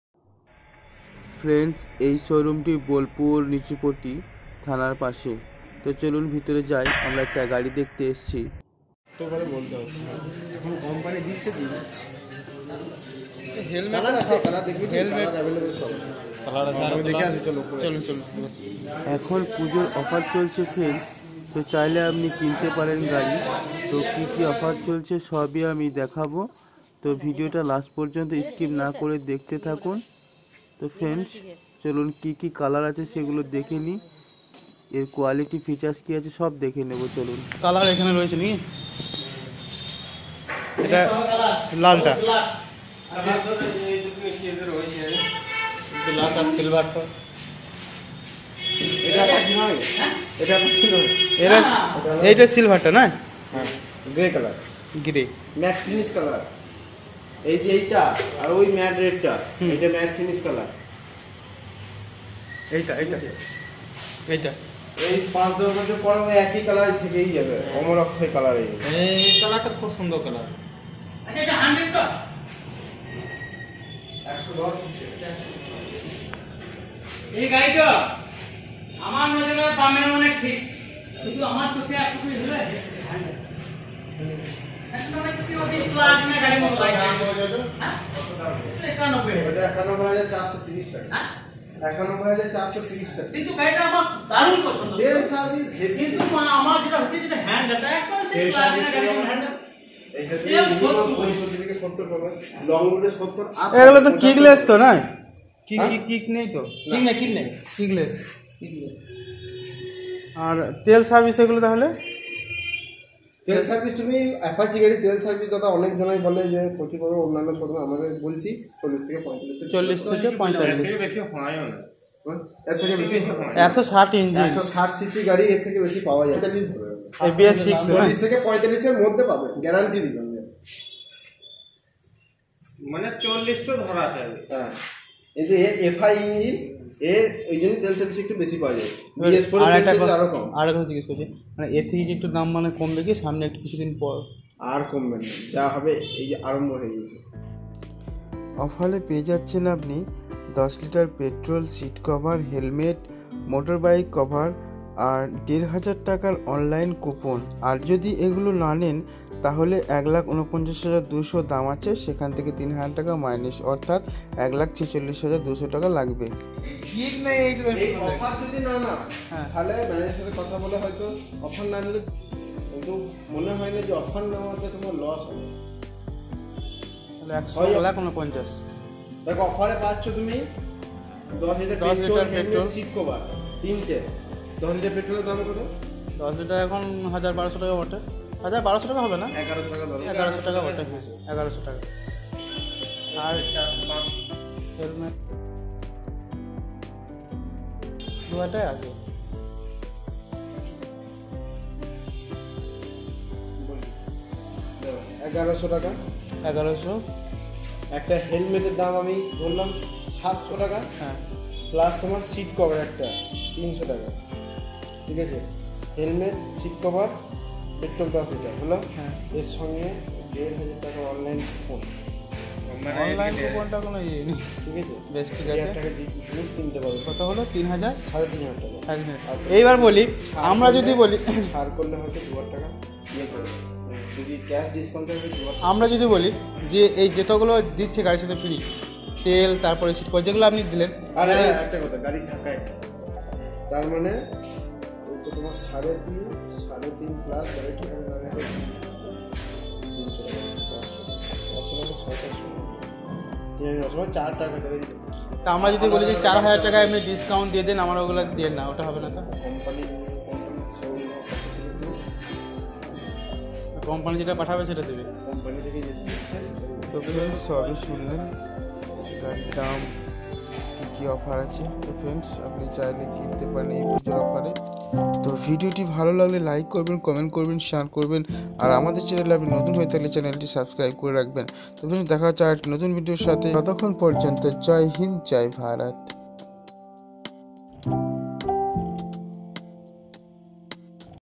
Apache RTR 160 V4 2022।সুযোগ হাত ছারা করবেন না।পুজোর ধামাকা অফার।
21/09/22-এই পুজোর সুময়ে,
Apache RTR  160 v4 motor byke টি যদি আপনি কেনেন,তাহলে অফারে পেয়ে যাবেন
((1)10 লিটার তেল,(2)হেলমেট(3)সিট কভার(4)byke ঢাকা cover (5)1500 টাকা অনলাইন কুপন।)জার মূল্য Total-4,000 টাকার মতো।
আর যদি এই গিফ্ট গুলো না নেন তাহলে,
3 হাজার টাকা ডিসকাউন্ট পেয়েযাবেন।
21/09/22 তারিখে বোলপুর মার্কেটে byke টির নেট দাম-1,49200।
গিফ্ট গুলো না নিলে (1,49200-3,000)= 1,46200 এই অফারটি nex 15 দিন পর্যন্ত থাকবে।

Hasibul Hasan.   From.    Birbhum. W.B.